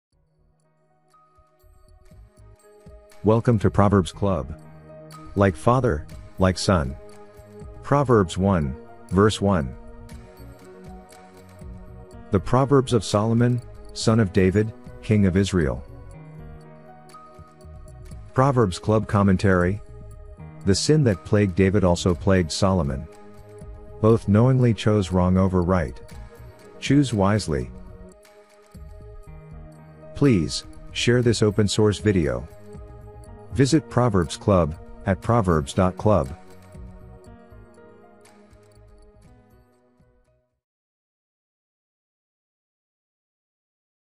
Like Father, Like Son - Proverbs 1:1
Welcome To Proverbs Club.
1) The proverbs of Solomon son of David, king of Israel:
Proverbs Club Commentary.
The sin that plagued David also plagued Solomon.
Both knowingly chose wrong over right.
Choose wisely.